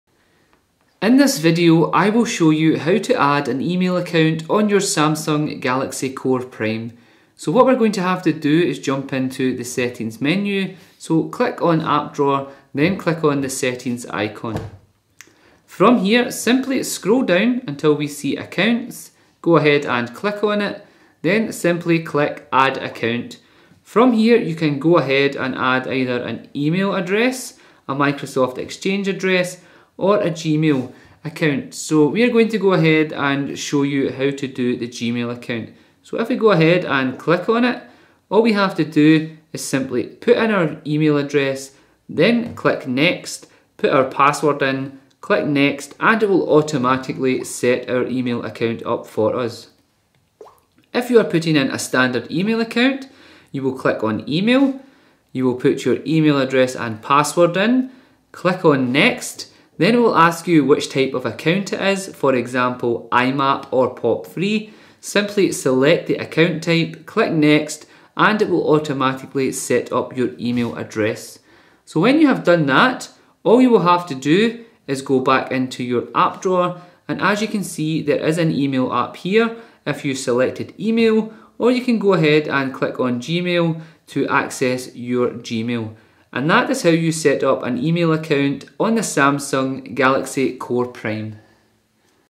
How To Add An Email Account - Samsung Galaxy Core Prime
A video how to, tutorial, guide on adding an email account on the Samsung Galaxy Core Prime.